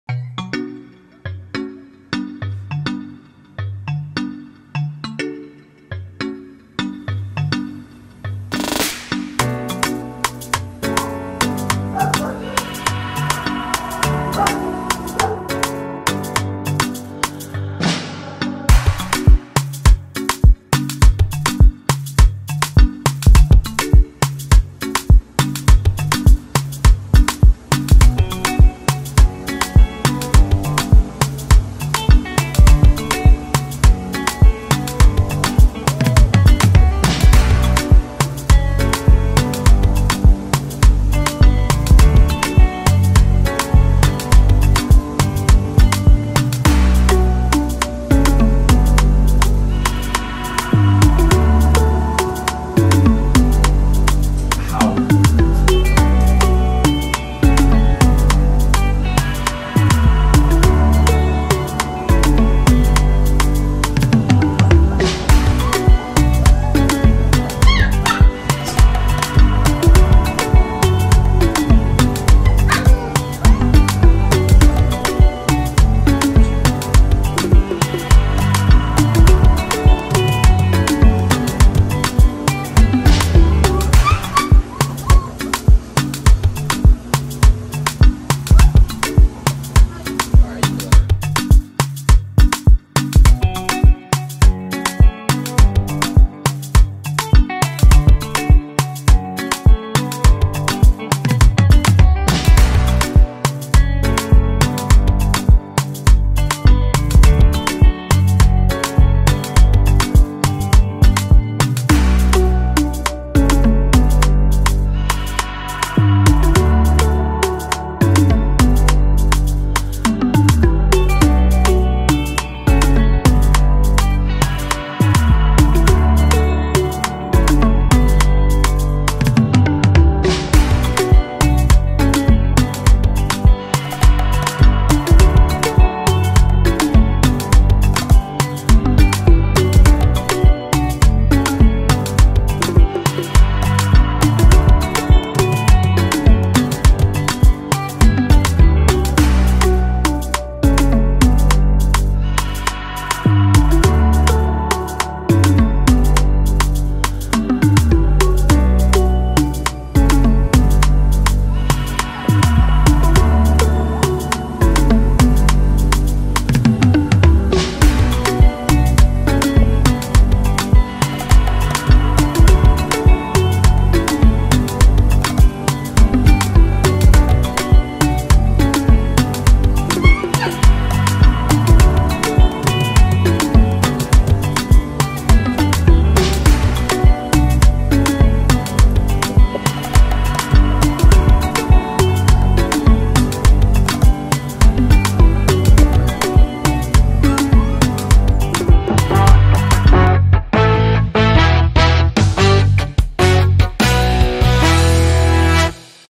AKC German Shepherd Puppies Born July 4th Now 4 Weeks Old
AKC German Shepherd Puppies